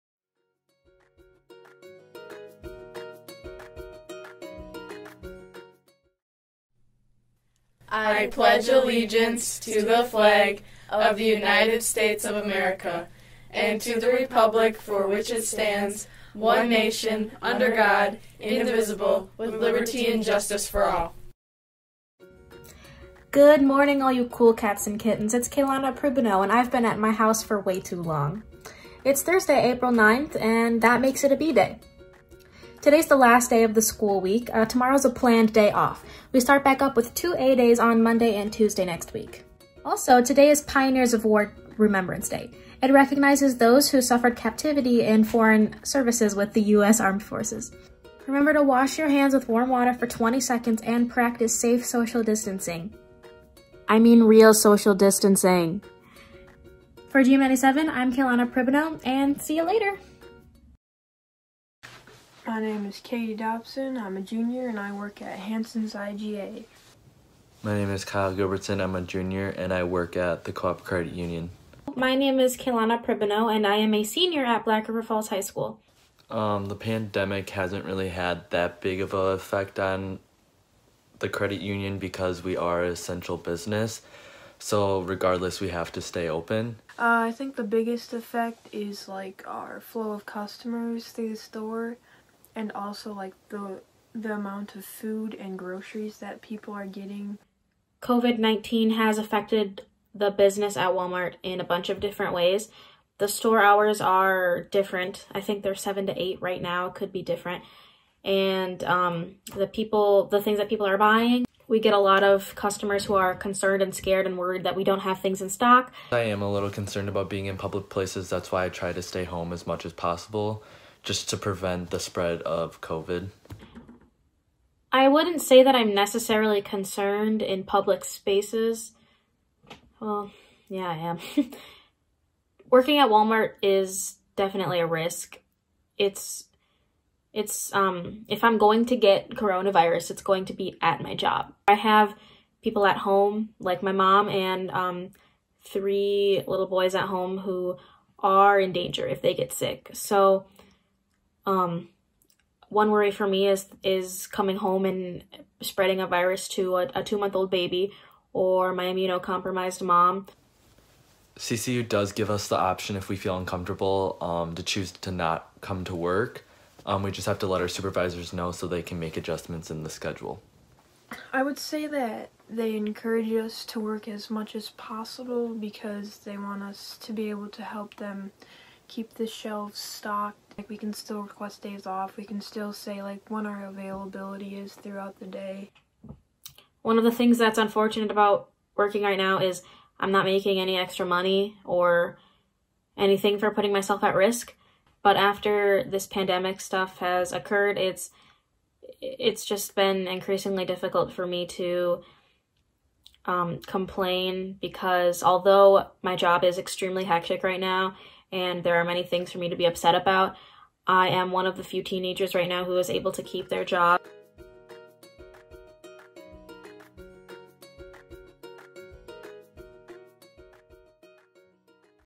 GM97: Announcements for April 9, 2020
Kaylana has this B-day and Ellie talks to Katie, Kyle and Kaylana about working in essential jobs.